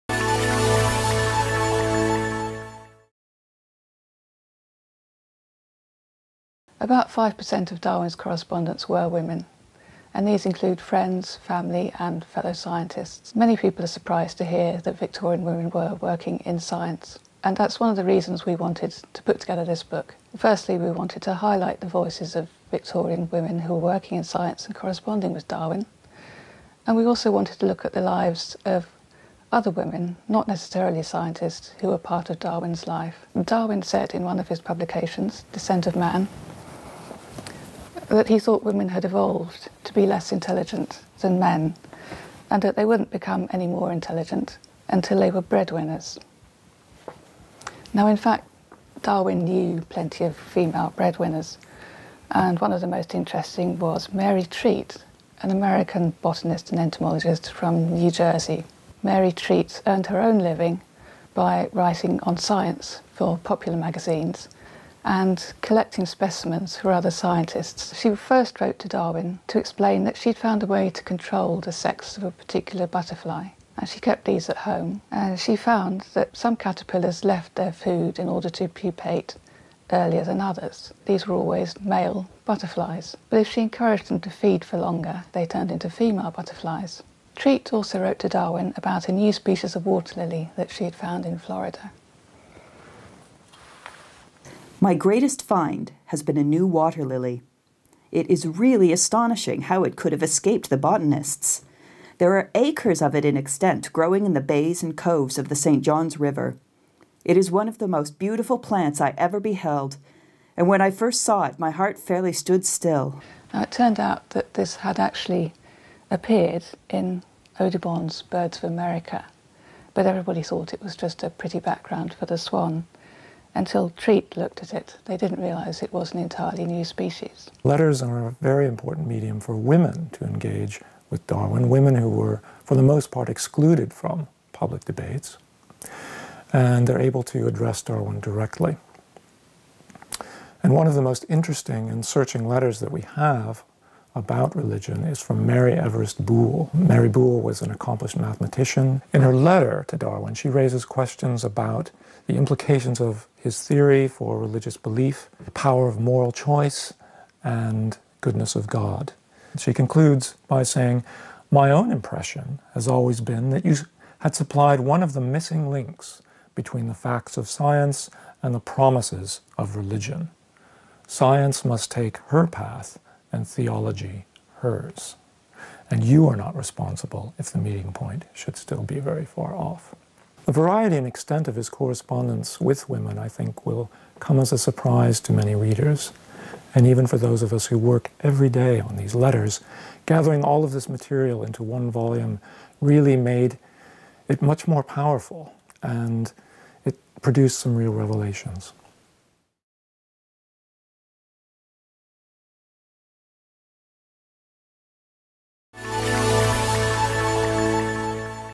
Darwin and Women: A Selection of Letters
Samantha Evans and Paul White discuss the recently published Darwin and Women: A Selection of Letters.

It includes a large number of hitherto unpublished letters between members of Darwin's family and their friends that throw light on the lives of the women of his circle and their relationships, social and professional, with Darwin. The letters included are by turns entertaining, intriguing, and challenging, and are organised into thematic chapters, including botany and zoology as well as marriage and servants, that set them in an accessible narrative context. Darwin's famous remarks on women's intelligence in Descent of Man provide a recurring motif, and are discussed in the foreword by Gillian Beer, and in the introduction. The immediacy and variety of these texts make this an entertaining read which will suggest avenues for further research to students.